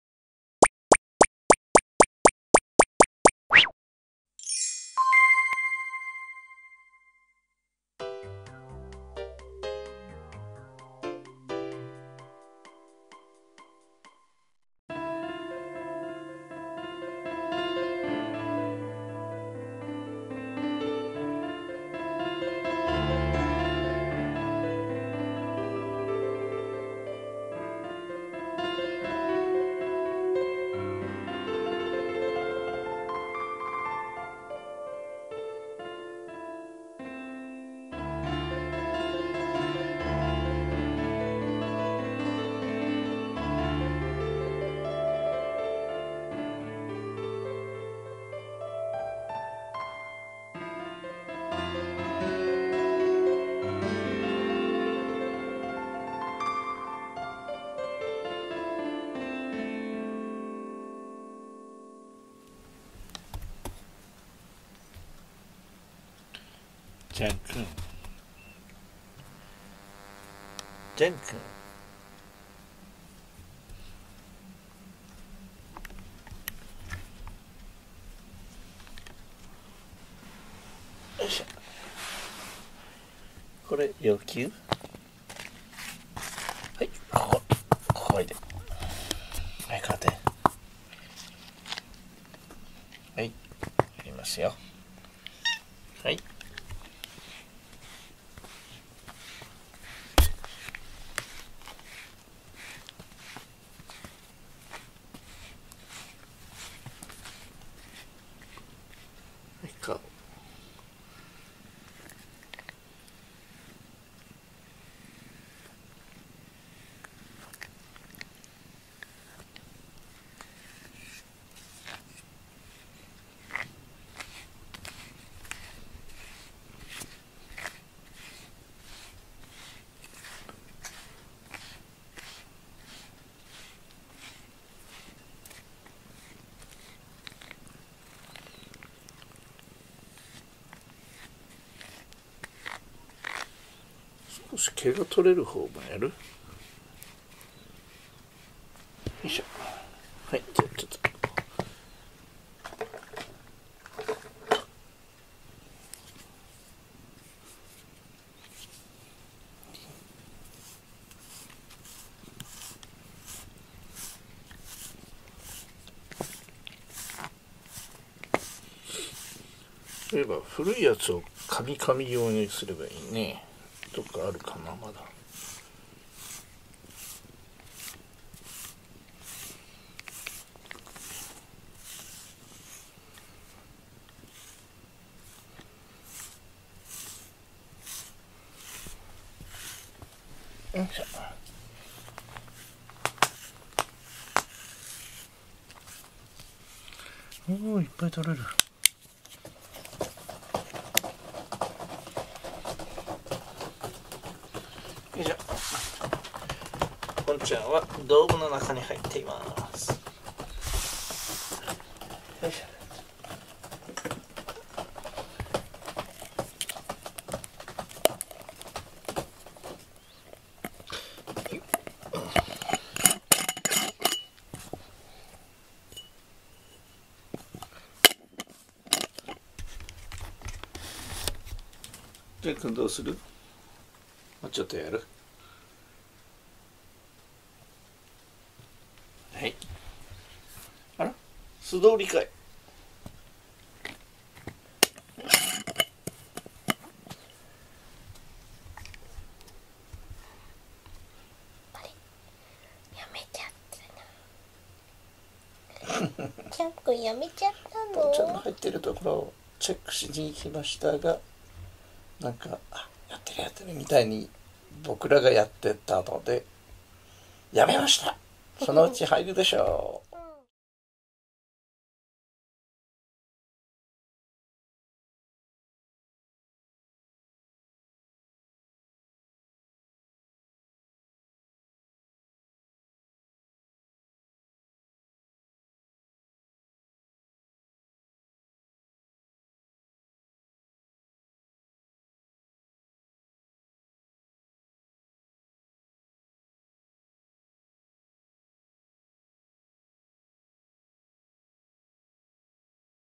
とても貴重な「ねこのきもち」付録のラバーブラシ【Jean & Pont 2319】2020/11/28
我が家では１０年前から愛ようしているラバーのブラシは雑誌の「ねこのきもち」の付録としてついてきたもので、手にはめることで当時の凶暴だったジャンくんの背中を素手で触るまでの擬似ナデナデとしてとてもありがたいものでした。
その後お店などで探しましたがちょうどいいものがなく、かなり古くなってトゲが丸く削られたものを使っていたりしました。
以前にもプレゼントとしていただいたことがあるのですが、今回もポンちゃんの誕生日のお祝いでいただき本当にうれしいです。
大事に使わせていただきます。

高画質　A4タテ　ジャンくんとポンちゃん

低画質　A4タテ　ジャンくんとポンちゃん